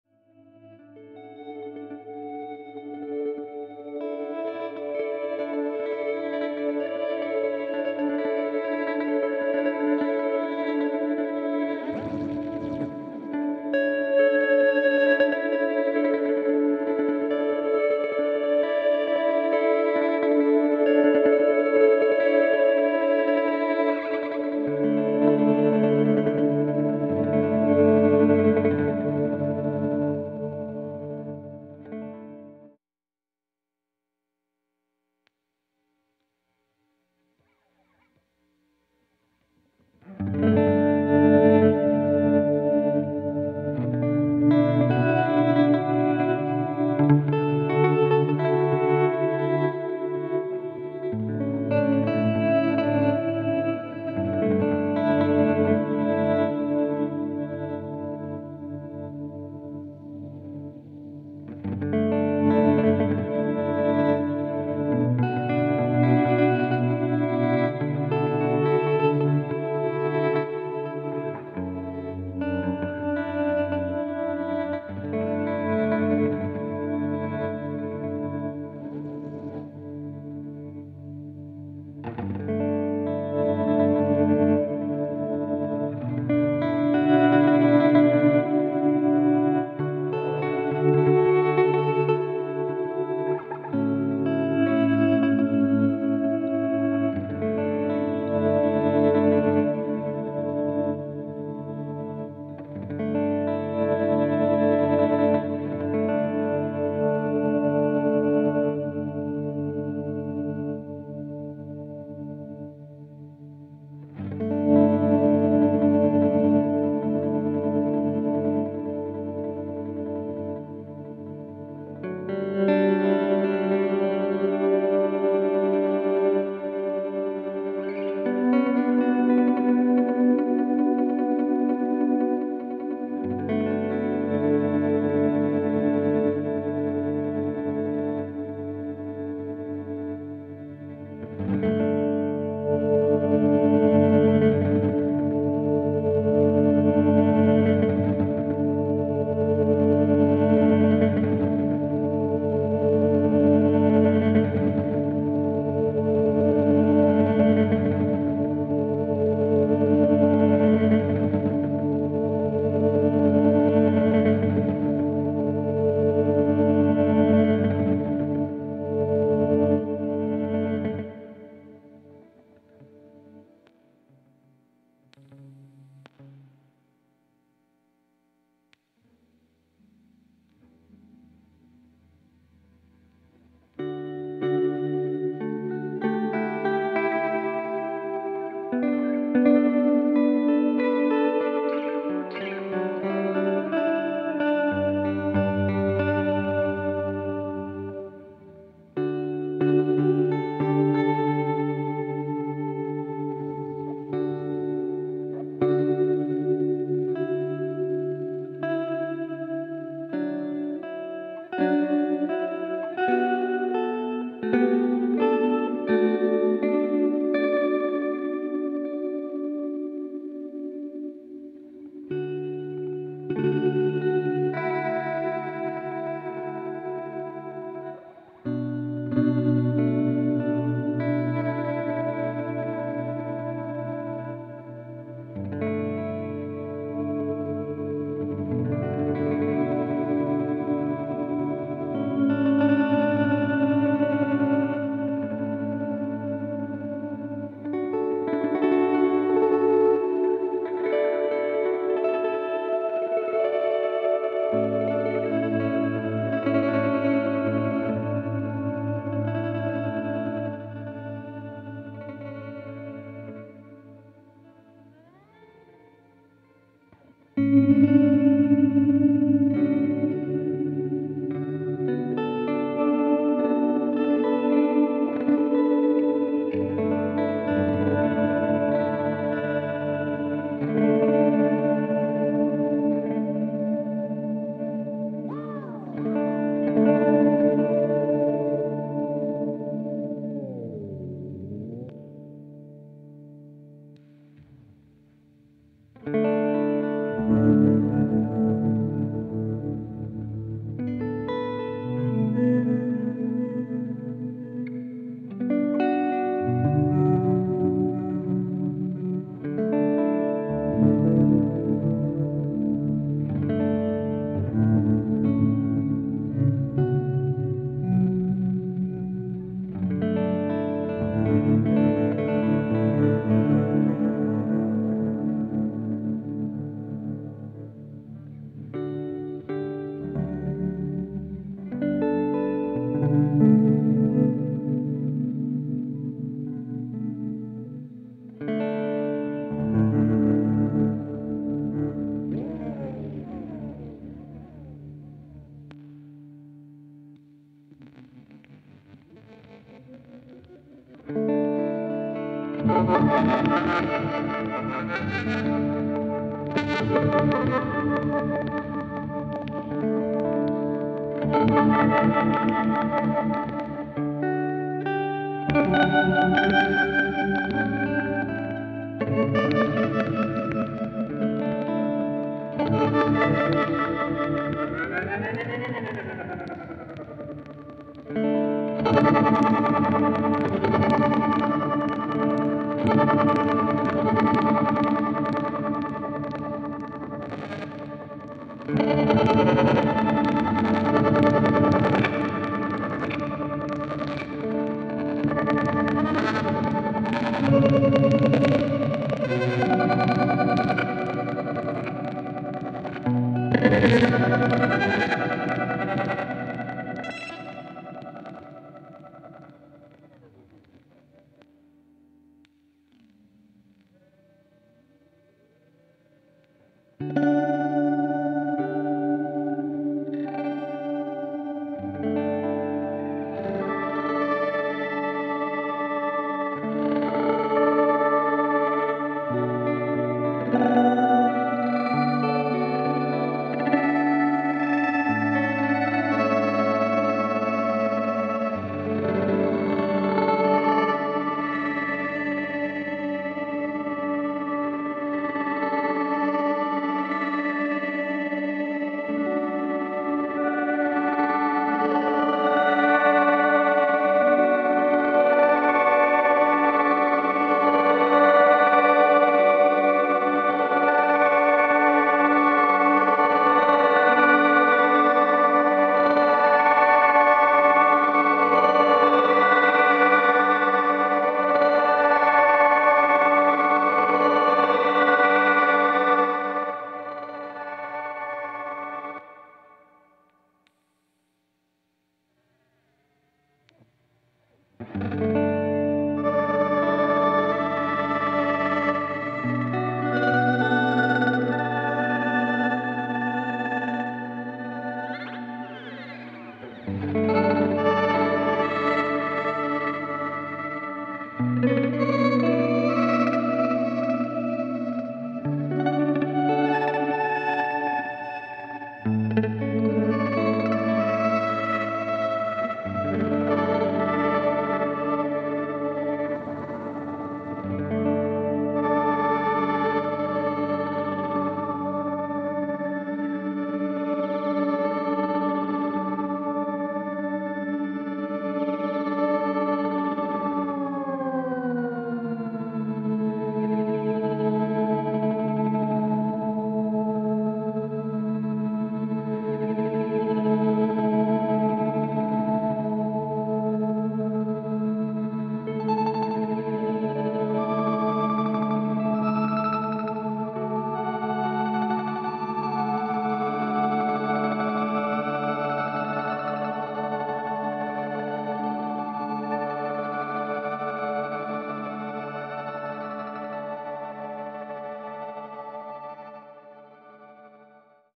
Pladask Elektrisk Bakfram - Demo
I've been having a lot of fun with this delay/time-stretcher/looper from Pladask Elektrisk so I thought I'd do a quick (well, actually 9 mins long) walk-through of some of the controls and sounds you can get from it!  Let me know if you have any questions below in the COMMENTS.

Signal was:
Fender Jazzmaster - Bakfram - Neunaber Immerse (W3T algo) - Strymon Iridium.